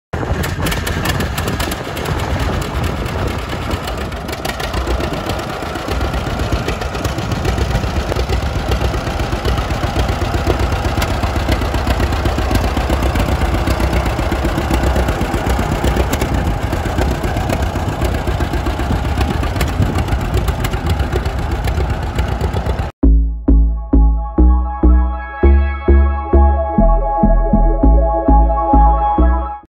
Lot 86, 1951 John Deere A Tractor
Selling as part of the 1st Annual RES Vintage Power, Sign & Toy Auction on Saturday, October 17, 2020, at 9 AM at the RES Auction Services Equipment Yard at 375 Fry Road, Wooster, Ohio.
or to consign call (833) SOLD-RES.